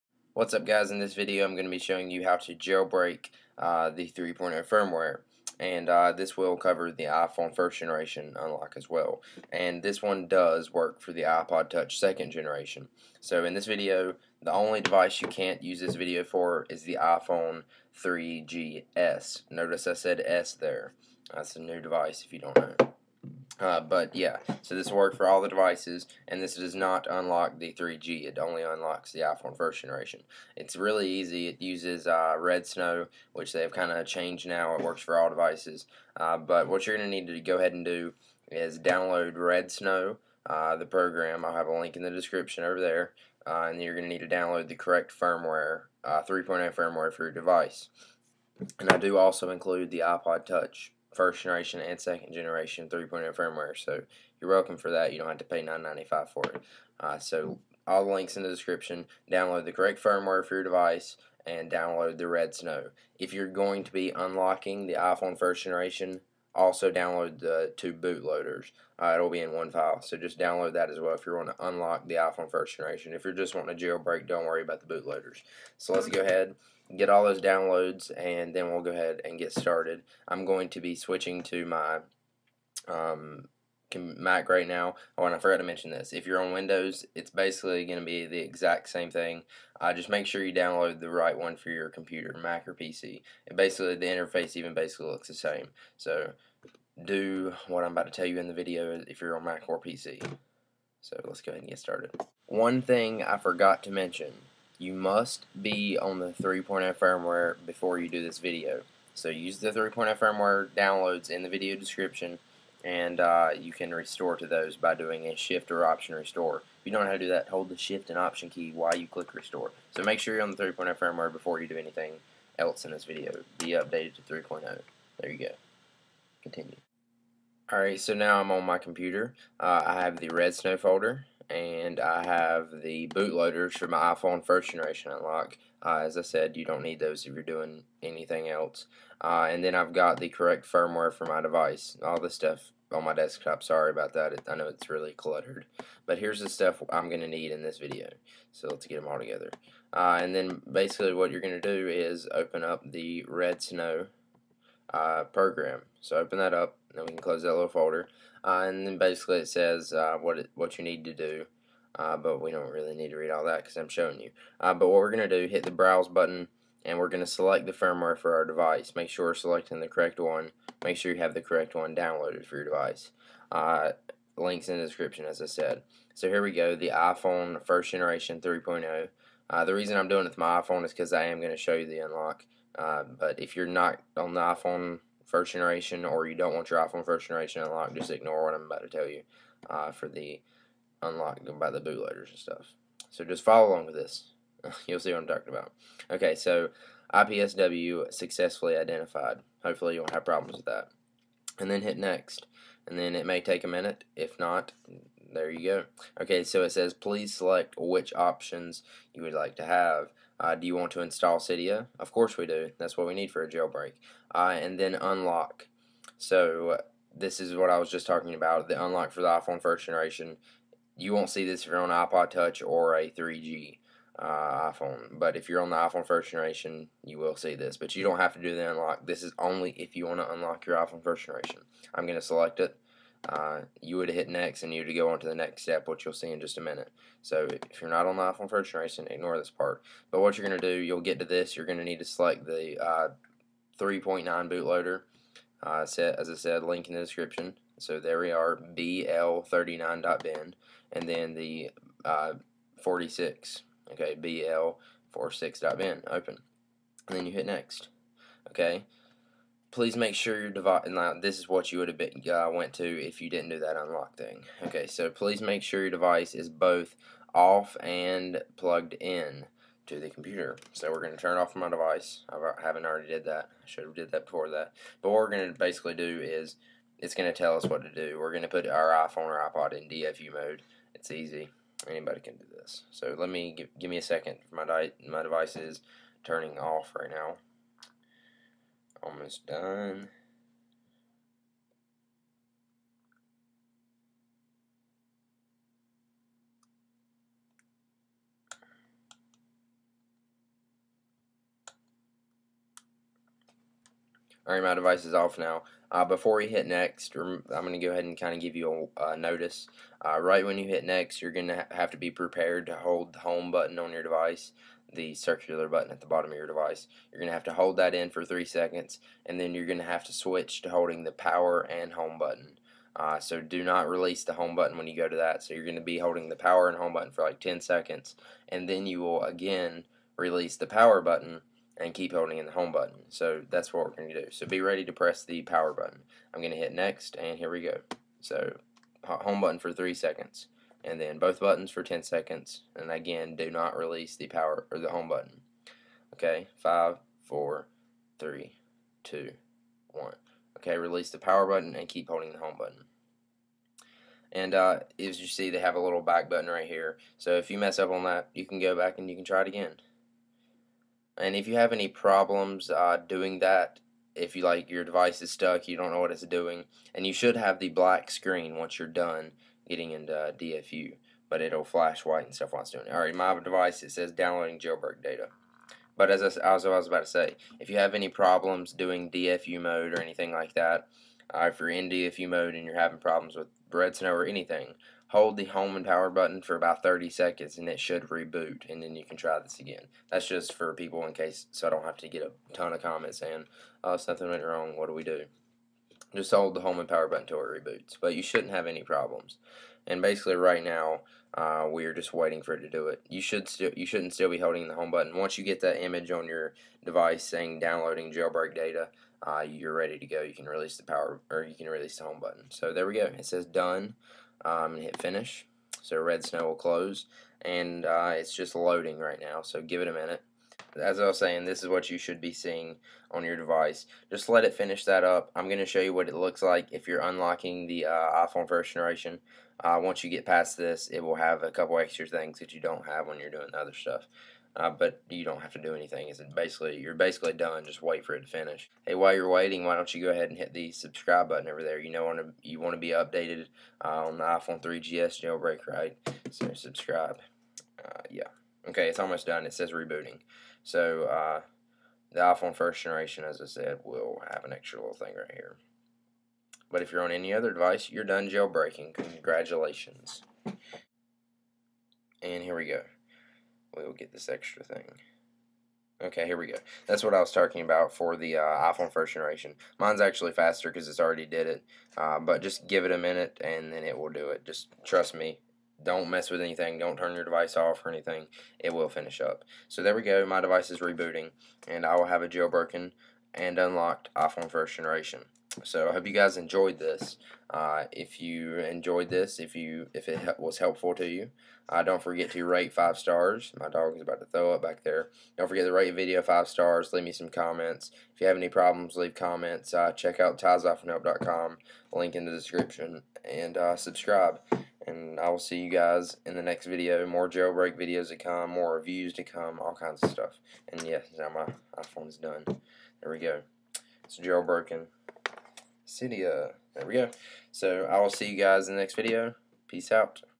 Jailbreak 3.0 Firmware iPhone or iPod Touch
In this video I show you how to jailbreak all devices on the 3.0 firmware excluding the iPhone 3G S. This works on XP and Vista, but only intel Macs. If your computer changes the .ipsw file to a .zip just rename .zip to .ipsw. Also you can try using another browser other than Safari.